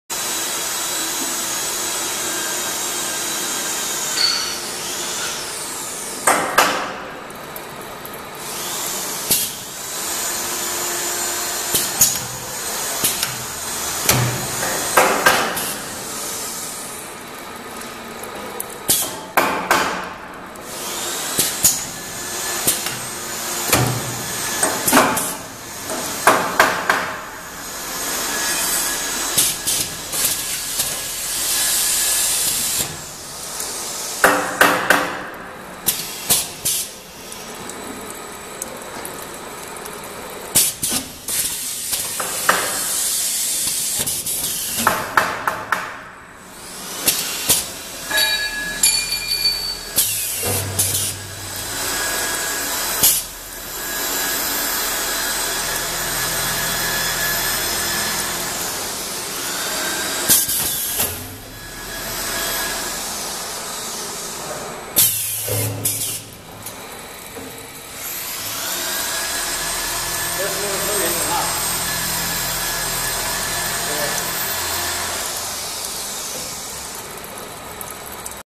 Wuxi KingPack Automatic Weighing Type Bucket Filling Machine for Paint Coating
Contact name: Sunny
WUXI JINGPAI MACHINERY CO., LTD